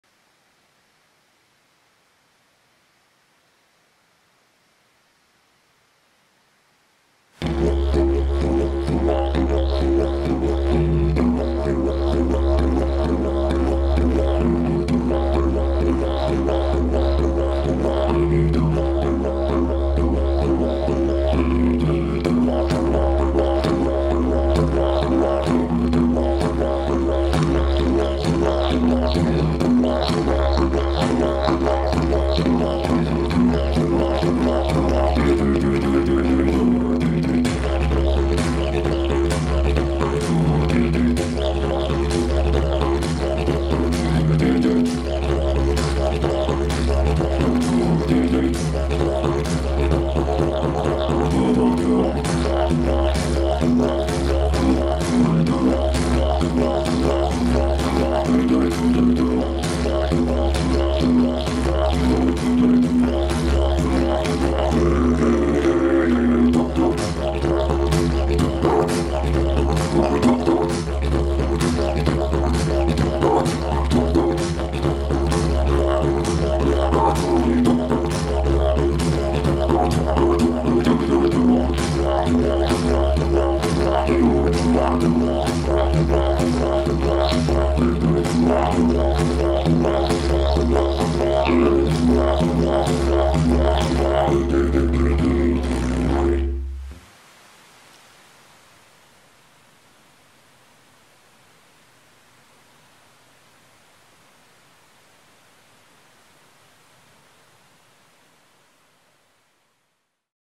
Didgeridoo solo #17 / Shigeki Fukuda
I play didgeridoo in nature.

I want to share with you.